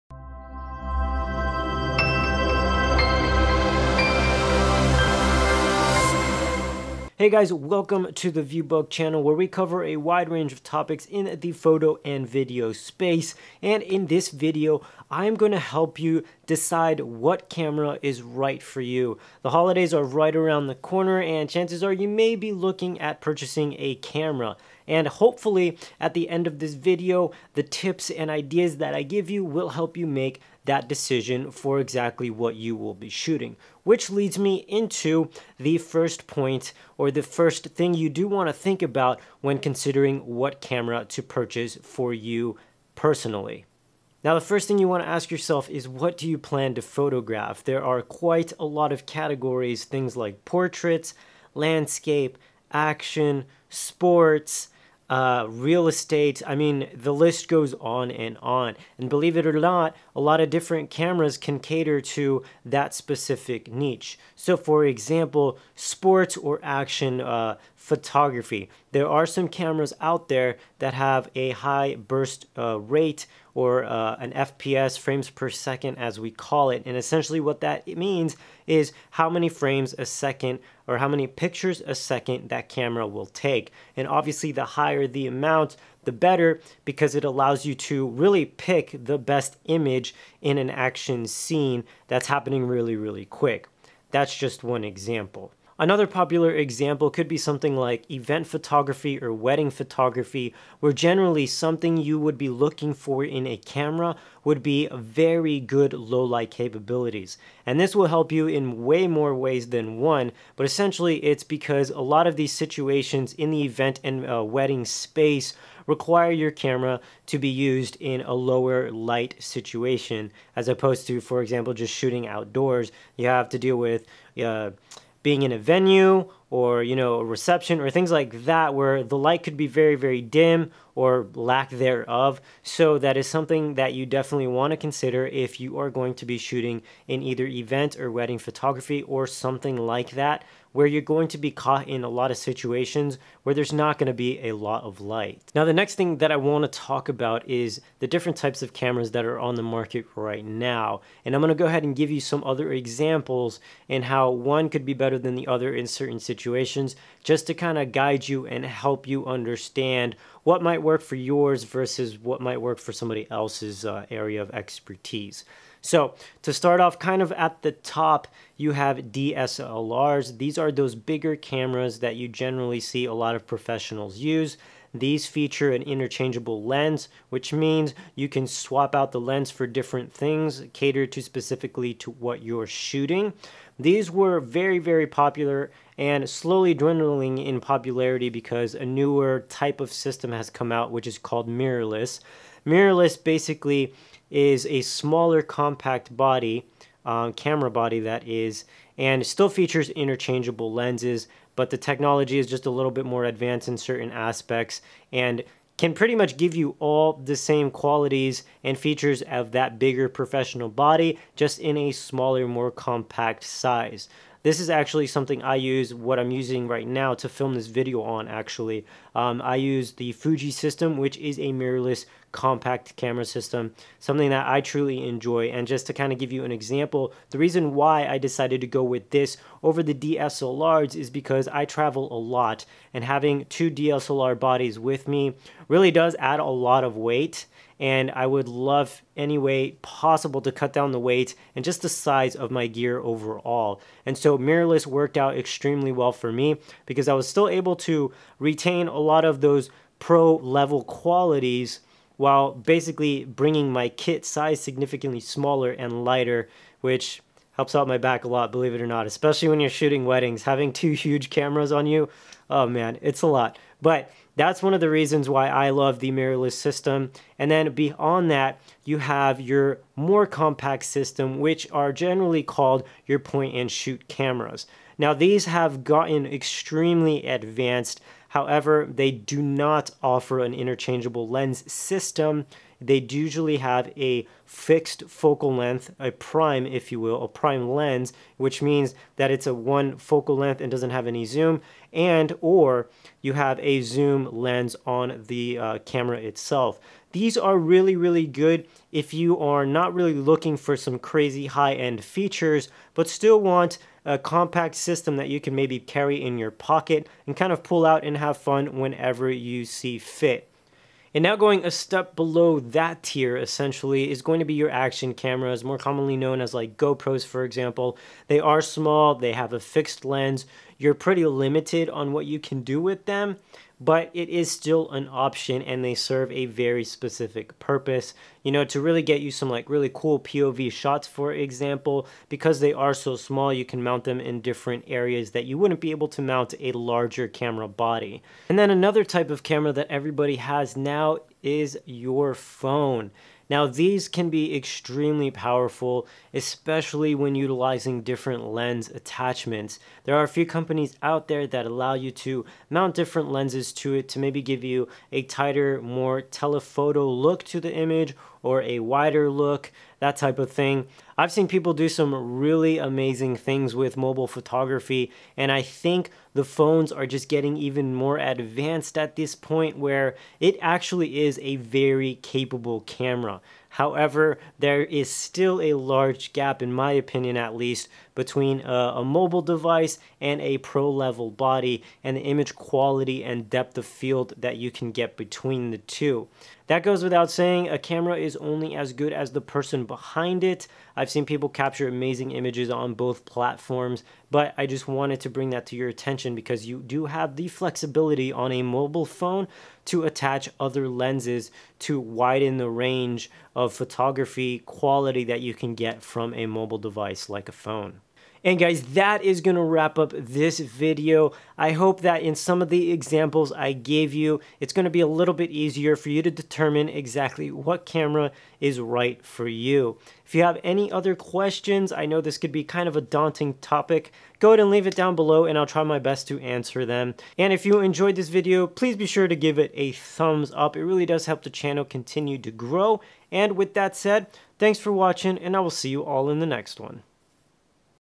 What Camera To Buy? Mirrorless vs. DSLR 📸 - Camera Buying Guide
With the holidays fast approaching, you may be looking to purchase a camera for yourself or someone dear to you. If so, you're in luck because in this video, Ace Noguera breaks down the different types of digital cameras and guides you on choosing the right camera for your specific needs and price range.

Do you have any camera buying tips to add?

Participate in the coolest opportunities with chances at fame and more

Share your best photos and collaborate with other creators like you